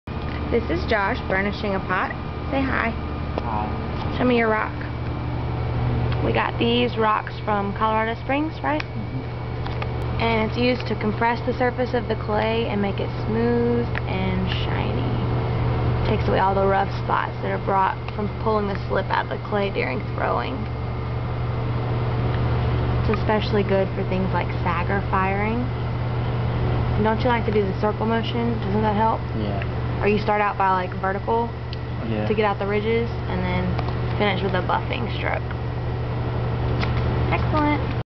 Bursishing stoneware
josh burnishes one of his first pieces with a stone from colorado springs.